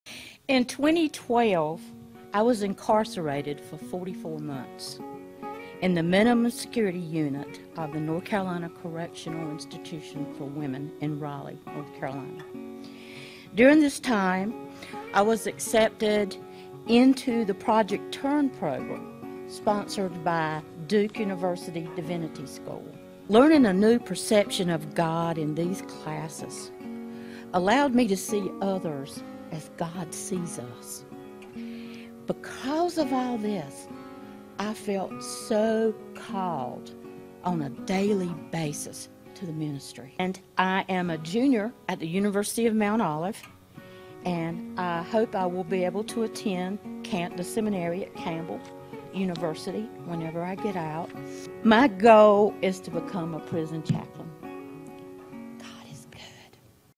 A Look at Why We Do What We Do | Kaye Ward, Duke University Divinity School
Featuring Duke University Divinity School alum testimonial footage from the 2018 ATS/COA Biennial Meeting in Denver, Colorado.